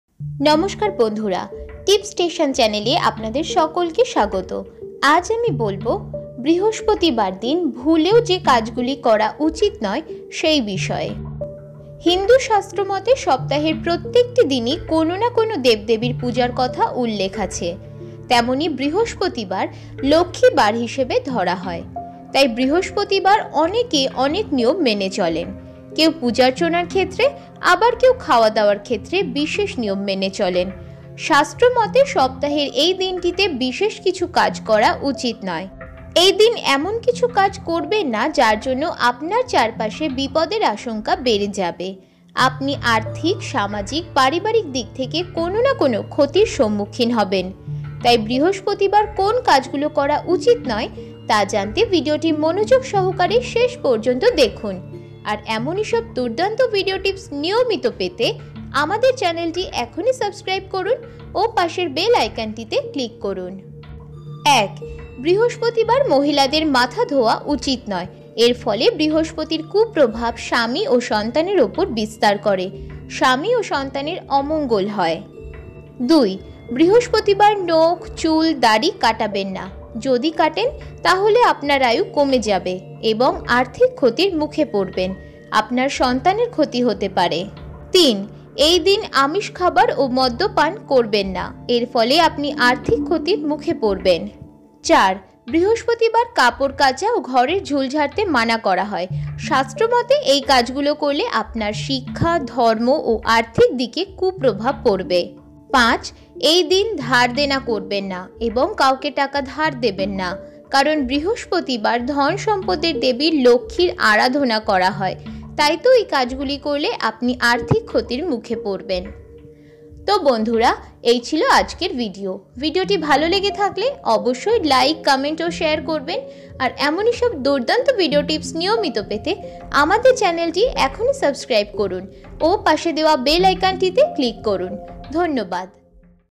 বৃহস্পতিবার দিন ভুলেও এই ৫টি কাজ করবেন না, মা লক্ষ্মীর কোপে ভিখারী হয়ে যেতে পারেন..
হিন্দু শাস্ত্র মতে সপ্তাহের প্রত্যেকটি দিনই কোন না কোন দেব দেবীর পূজার কথা উল্লেখ আছে। তেমনি বৃহস্পতিবারকে লক্ষ্মী বার হিসেবে ধরা হয়। তাই বৃহস্পতিবার অনেকে অনেক নিয়ম মেনে চলে। কেউ পূজো অর্চনা ক্ষেত্রে আবার কেউ খাওয়ার দাওয়ারের ক্ষেত্রে বিশেষ নিয়ম মেনে চলে। শাস্ত্র মতে সপ্তাহের এই দিনটিতে বিশেষ কিছু কাজগুলো করা উচিৎ নয়। এই দিন এমন কিছু কাজ করবেন না যার জন্য আপনার চারপাশে বিপদের আশঙ্কা বেড়ে যাবে। আপনি আর্থিক, সামাজিক, পারিবারিক দিক থেকে কোন না কোন ক্ষতির সম্মুখীন হবেন। তাই বৃহস্পতিবার কোন কাজ গুলো করা উচিৎ নয় তা জানতে ভিডিওটি মনোযোগ সহকারে শেষপর্যন্ত দেখুন আর এমনই সব দুর্দান্ত ভিডিও টিপস নিয়মিত পেতে আমাদের চ্যানেলটি এখনই সাবস্ক্রাইব করুন ও পাশের বেল আইকনটিতে ক্লিক করুন।

Voice Credit - Poulami Bhattacharya